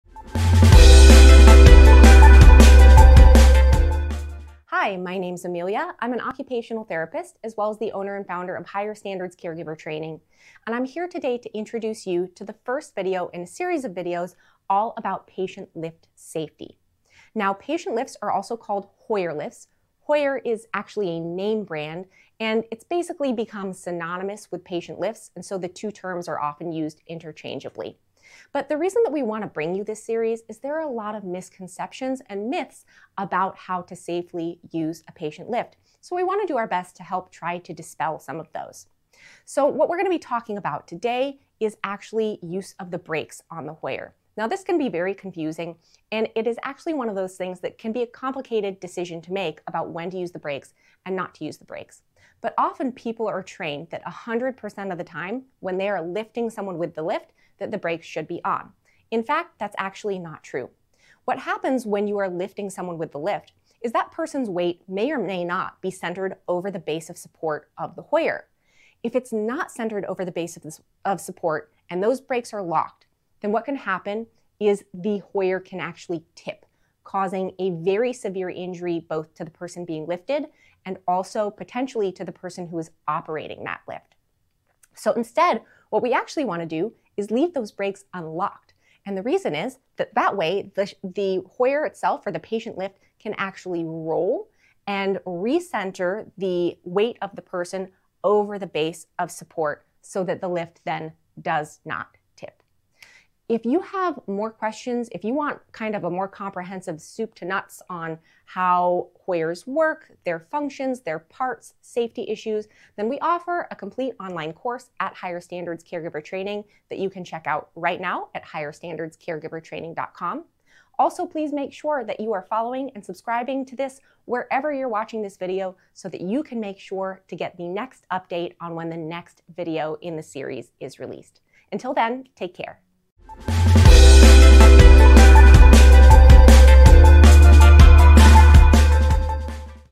Hoyer Lift Safety: Brakes!
One of the most common misconceptions about safe Hoyer use is over when to lock the brakes. Watch this quick video about brakes and safety during Hoyer use!